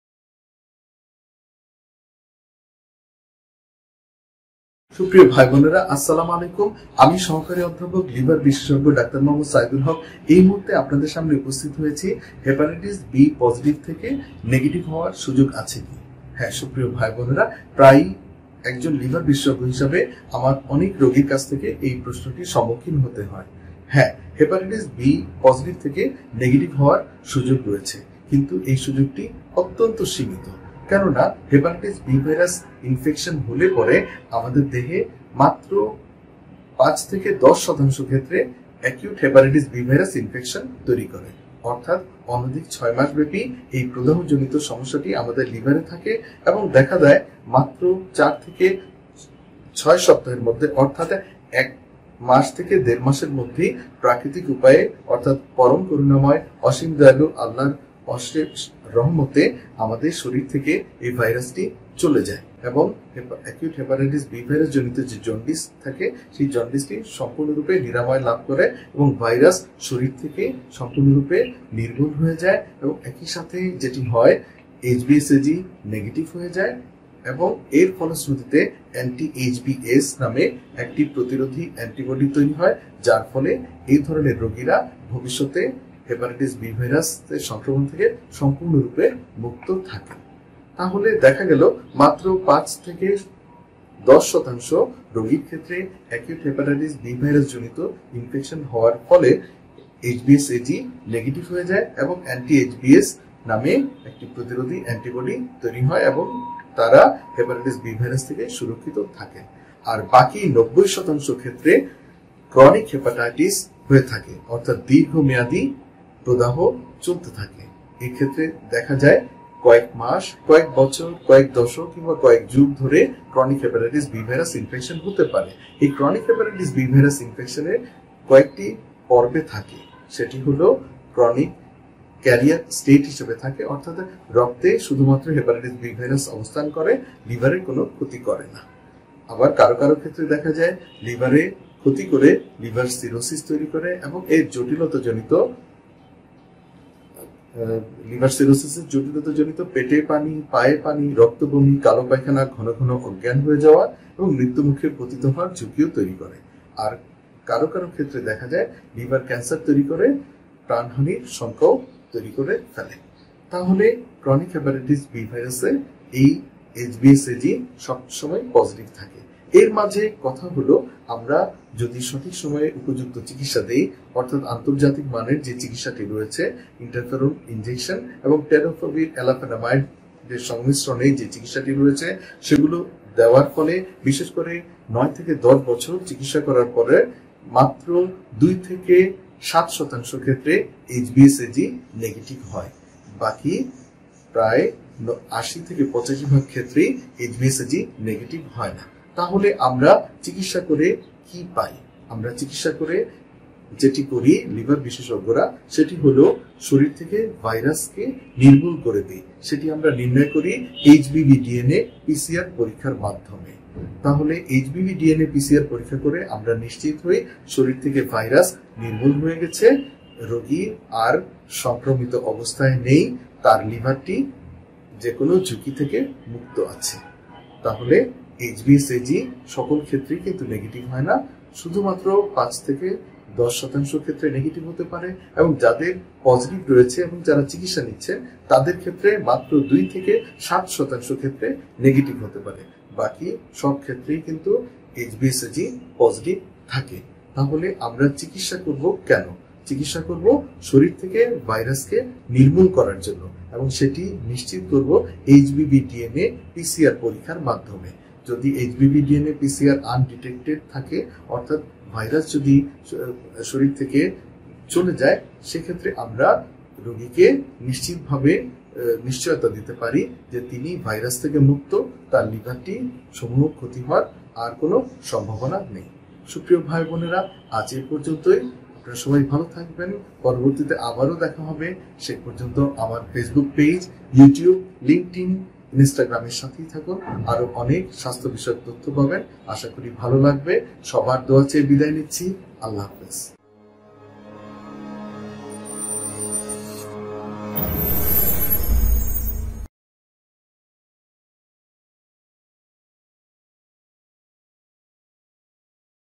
HBsAg পজিটিভ থেকে নেগেটিভ হওয়ার সুযোগ আছে কি | হেপাটাইটিস বি এর চিকিৎসা
ডাঃ এম. সাঈদুল হক, সহকারী অধ্যাপক, লিভার বিভাগ, ঢাকা মেডিকেল কলেজ হাসপাতাল, চিফ কনসালট্যান্ট, ঢাকা গ্যাস্ট্রো-লিভার সেন্টার